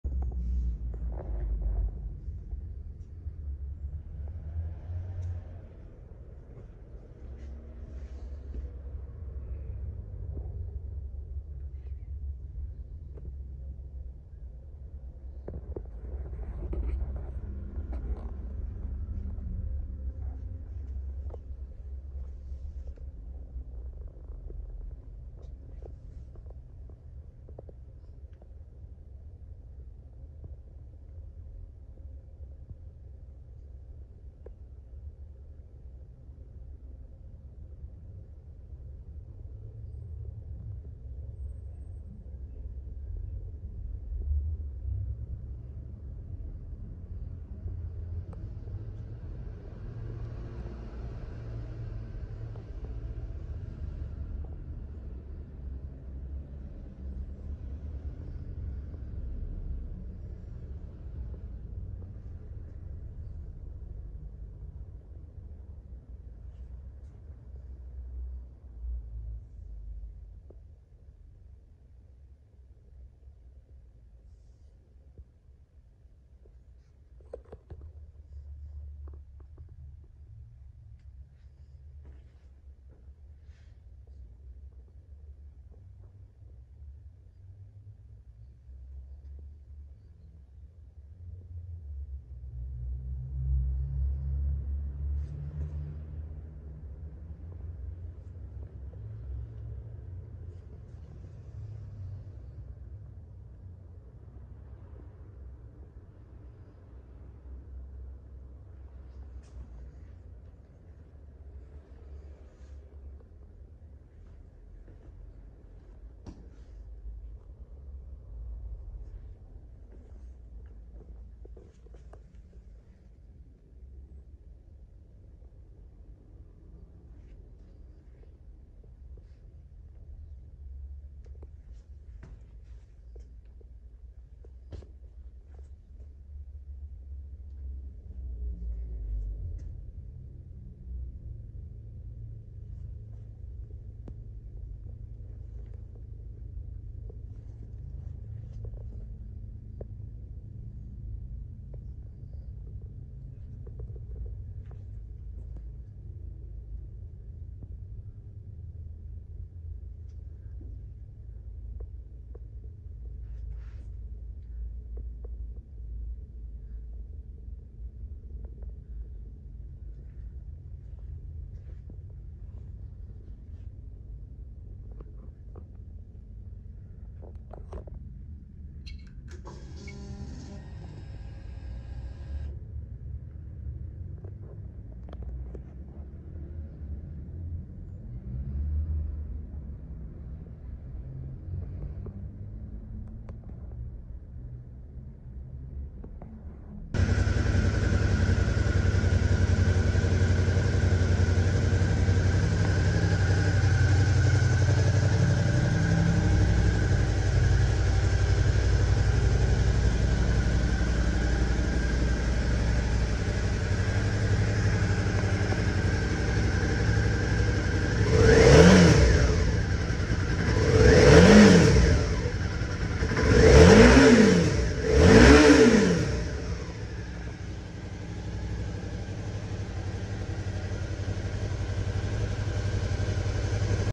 Suzuki V-Strom 650 XT 2021
Suzuki V-Strom 650 XT 2021, Vermelha, com protetor de motor, protetor de mãos, suporte para bauleto, controle de tração, freio ABS, manual e chave reserva, moto impecável e sem detalhes!!